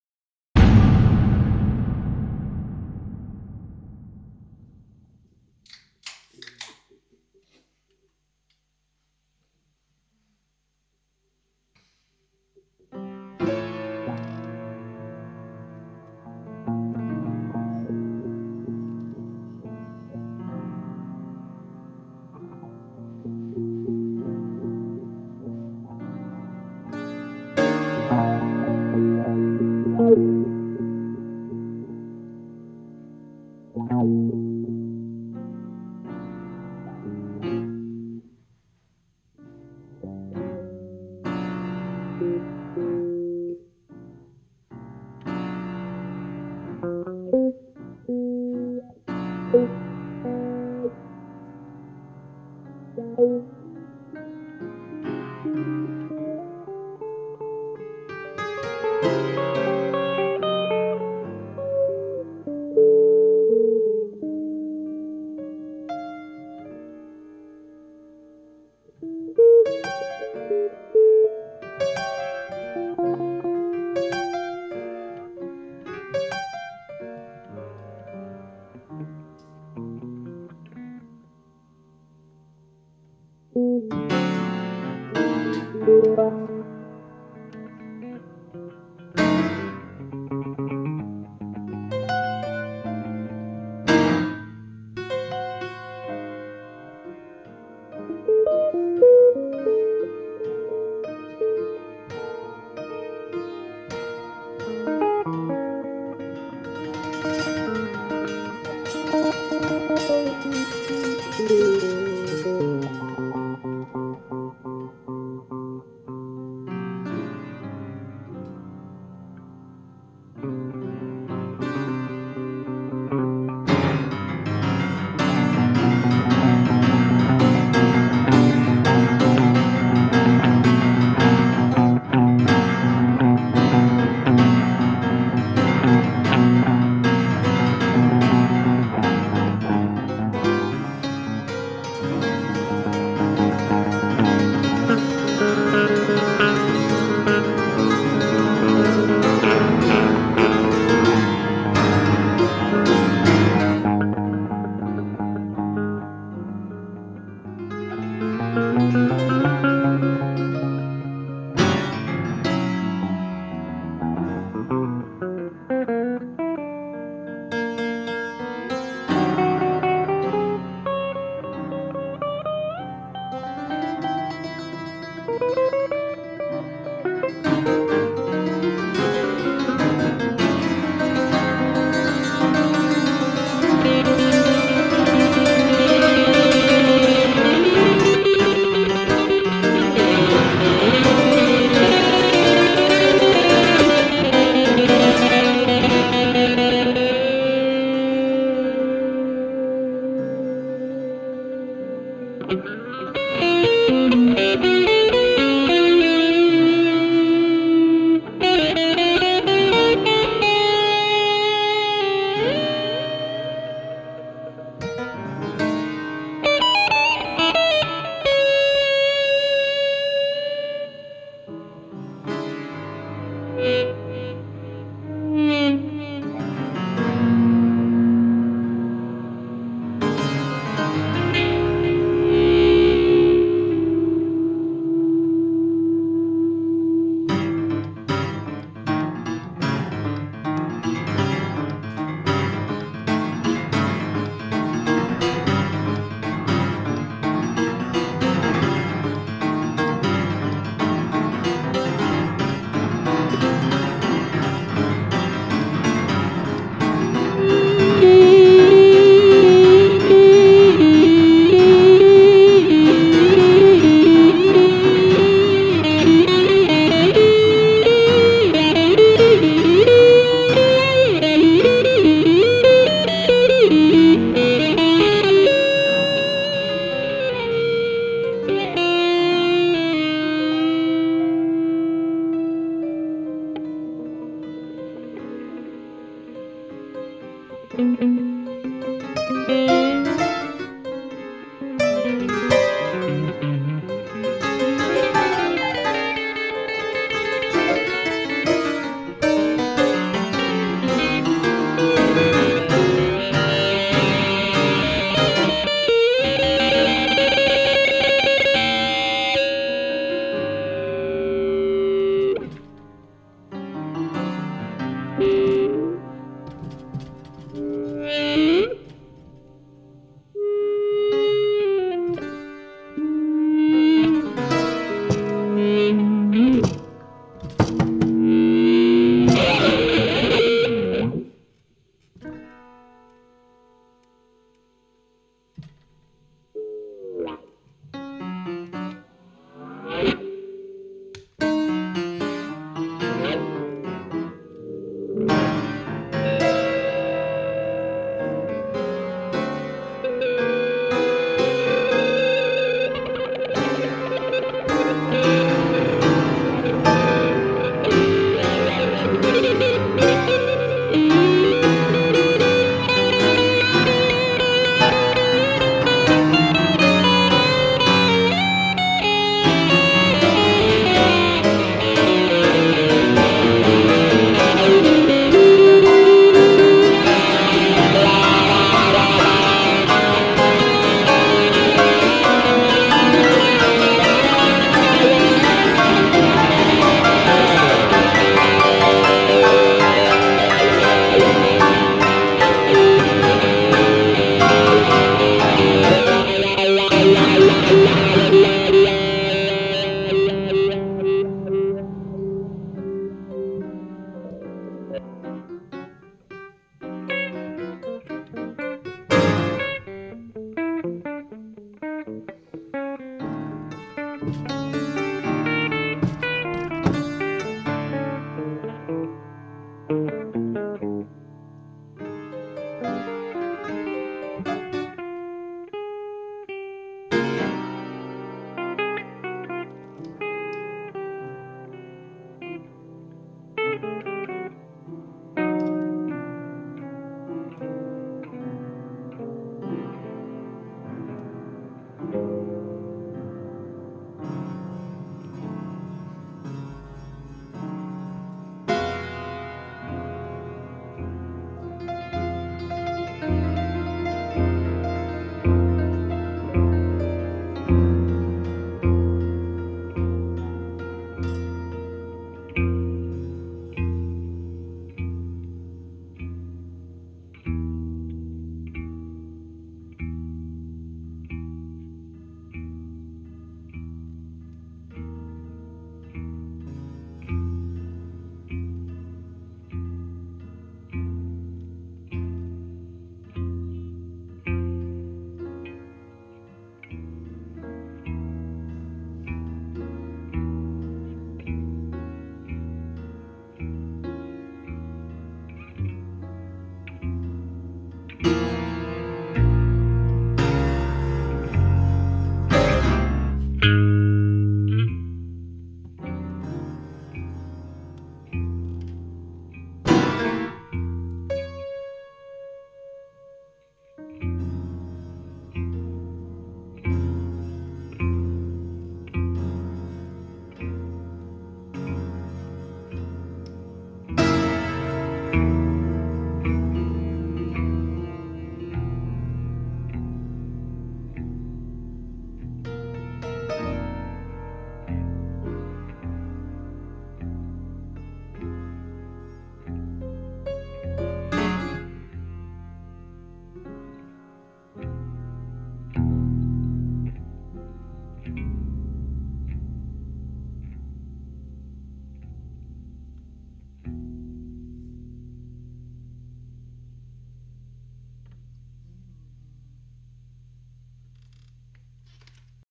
machin #40
improvisation bières / musique du 3 octobre 2012
Arnaud Juan : Guitare
Matthew Tyas : Piano